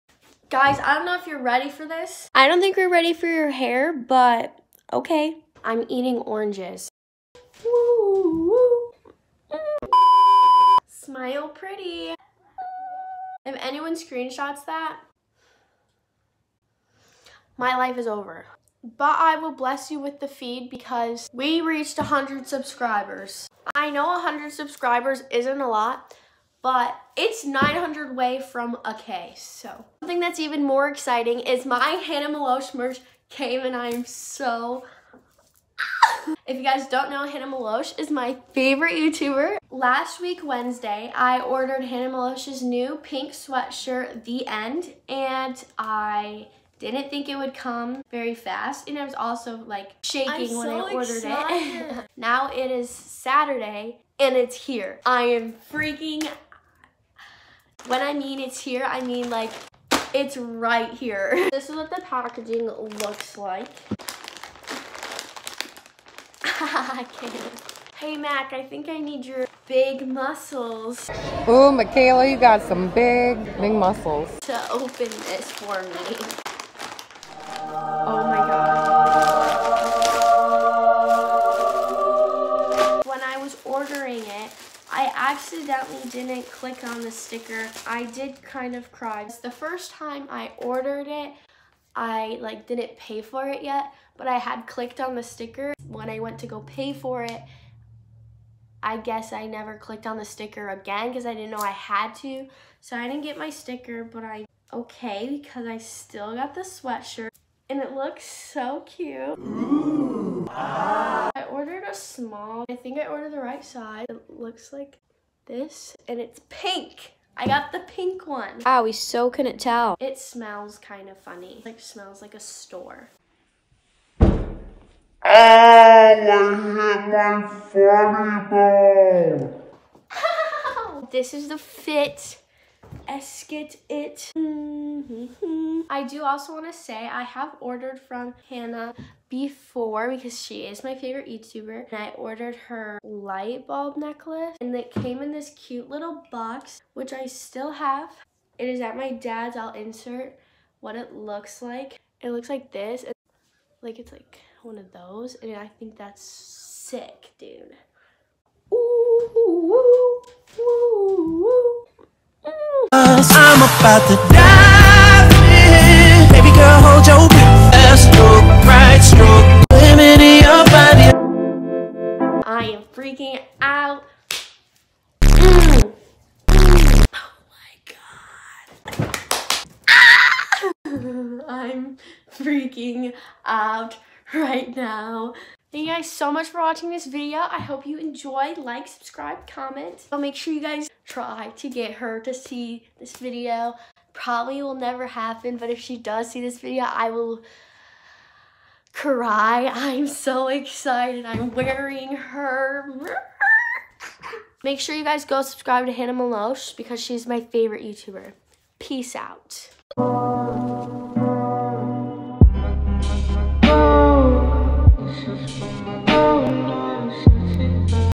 i got HANNAH MELOCHE merch| unboxing video
I am very thankful I was able to order Hannah’s merch! I am grateful I was able to help and support her by buying from her!
It is my dream to meet her because she seems like the coolest person in the world! She motivates me to do YouTube and I enjoy her content!
Thank you guys for watching!! I hope you have a nice day :)